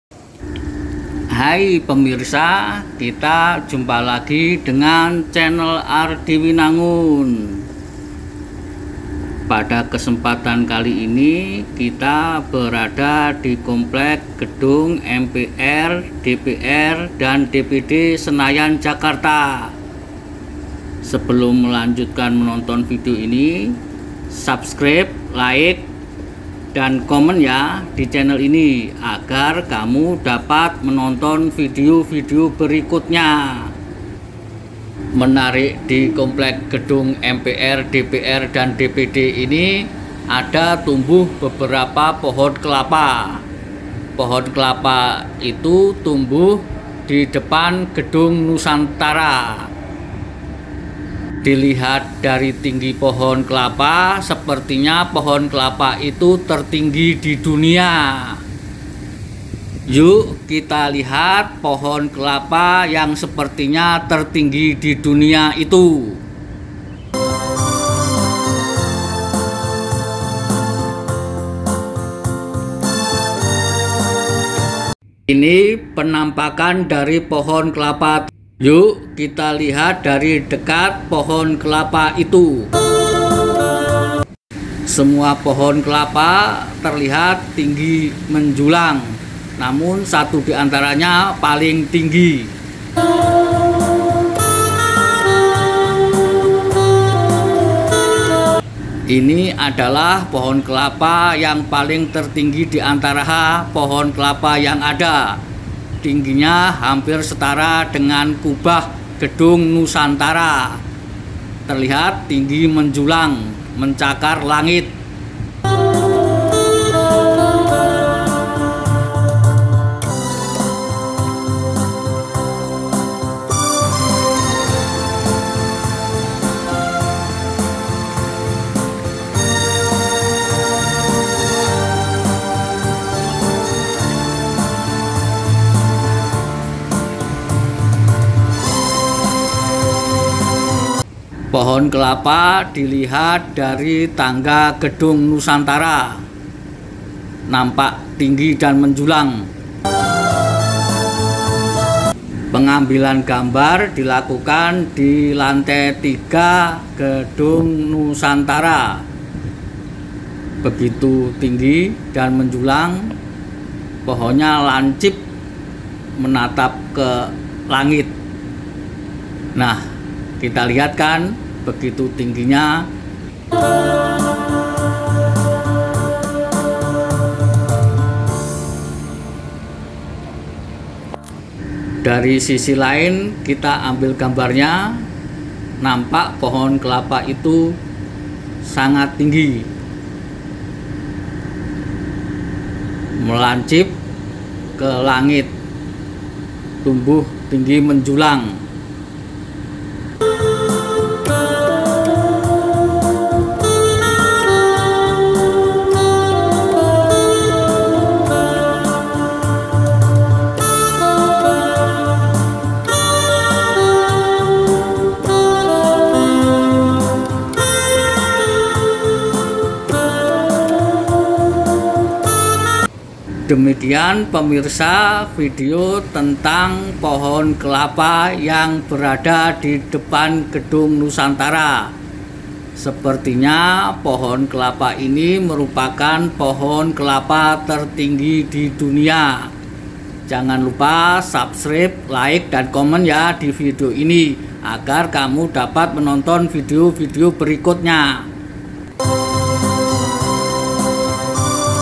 Sepertinya Ini Pohon Kelapa Tertinggi di Dunia
Di Depan Gedung Nusantara, Komplek Gedung MPR/DPR/DPD, Senayan, Jakarta, tumbuh beberapa pohon kelapa yang tinggi menjulang. Salah satu di antaranya paling menjulang sehingga bisa jadi ia merupakan pohon kelapa tertinggi di dunia. Bisa dikatakan pohon kelapa tertinggi dunia sebab ujungnya setara dengan kubah Gedung Nusantara.

Penasaran dengan pohon itu? simak saja pada video di atas.